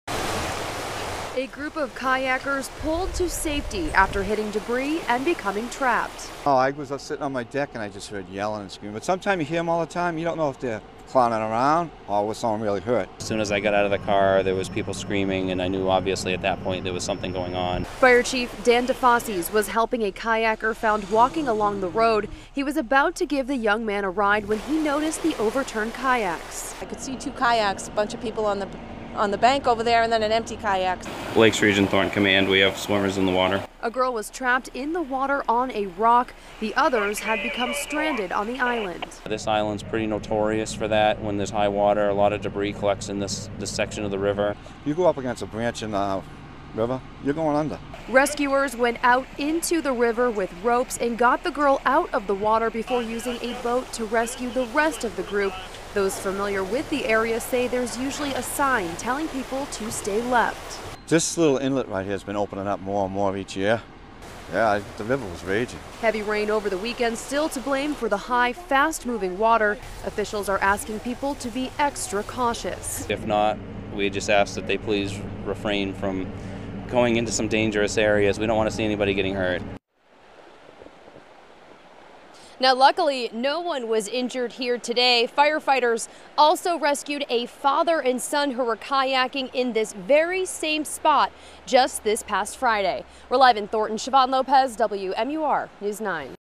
Several kayakers rescued from raging Pemigewasset River
Several kayakers were pulled to safety Wednesday after they got trapped in a dangerous section of the Pemigewasset River.